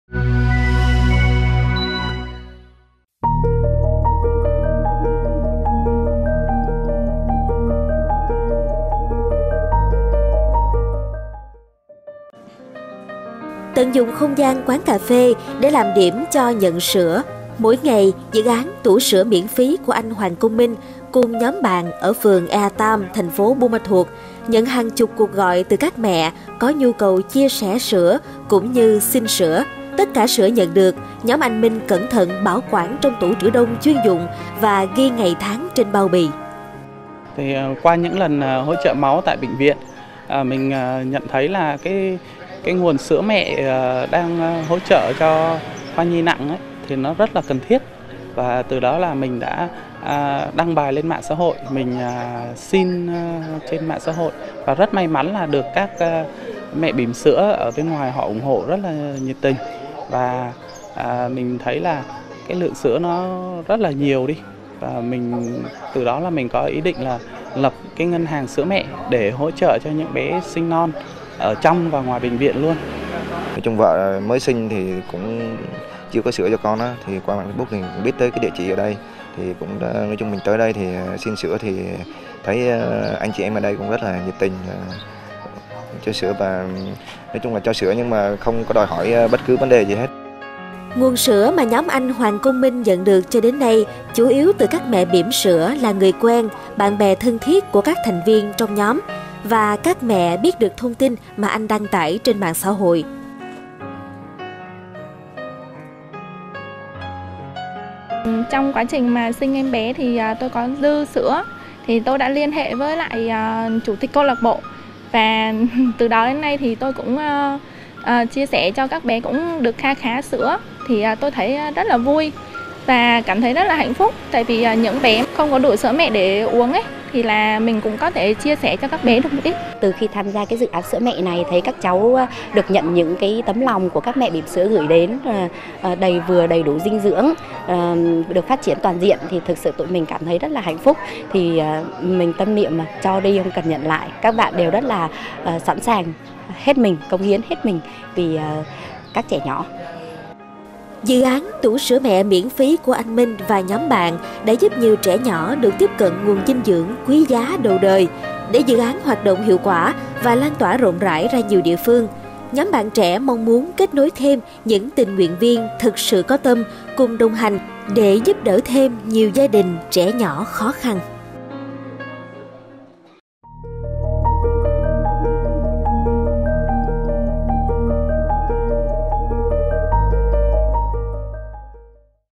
Cuộc thi Lan tỏa năng lượng tích cực 2023: Nguồn sữa yêu thương
Gần 3 năm đi vào hoạt động, dự án 'Tủ sữa mẹ miễn phí' của anh Hoàng Công Minh, cùng các tình nguyện viên tại TP Buôn Ma Thuột, tỉnh Đắk Lắk đã trở thành nhịp cầu lan tỏa sữa mẹ cho các bé sơ sinh sinh non, hoặc thiếu sữa mẹ trên địa bàn tỉnh.